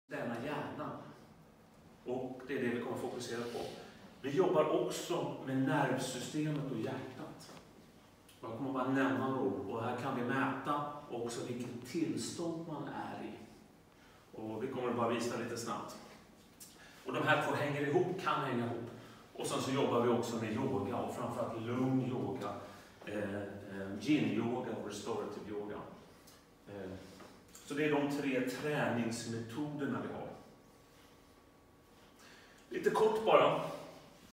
Föreläsning av FEEL it BETTER
Föreläsningar av FEEL it BETTER handlar om hur vi kan träna vår hjärna och nervsystem för att nå ett bättre tillstånd.